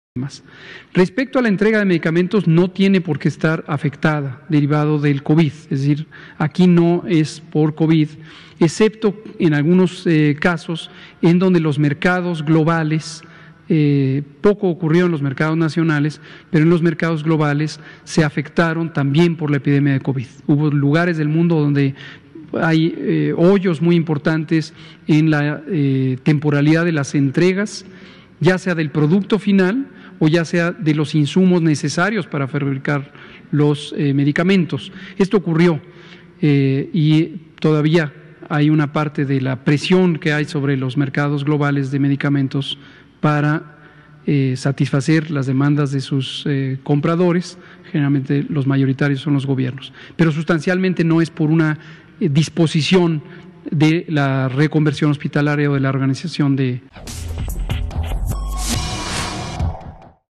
Mercados globales de medicamento se vieron afectados por la epidemia de COVID-19: López-Gatell
Hugo López-Gatell, Subsecretario de Prevención y Promoción de la Salud, explicó que los mercados nacionales de medicamentos no se vieron afectados por la epidemia, sin embargo, los mercados globales si se vieron aquejados por el virus.